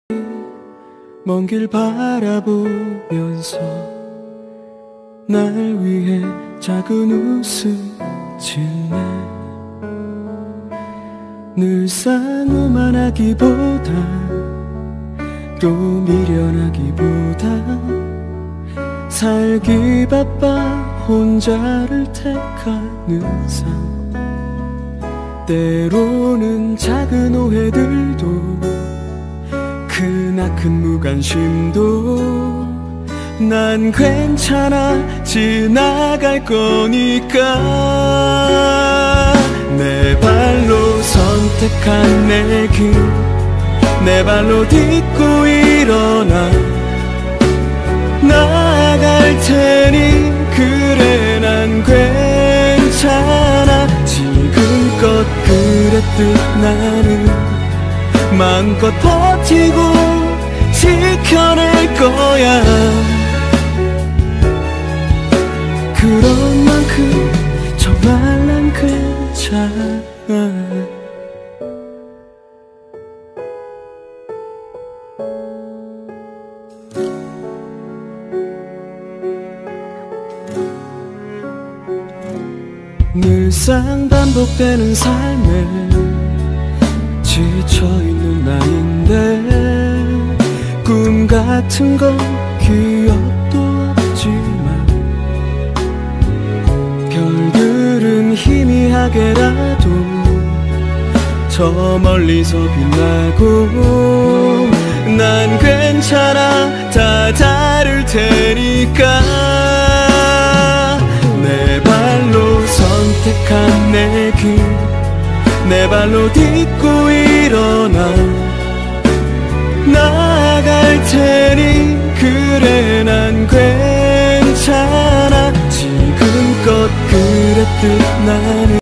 Jumin Han ( Mystic Messenger ) speedpaint ^^
Song By Yong Woo Shin
Art by me ( except for the background of the drawing ) background from ibispaintx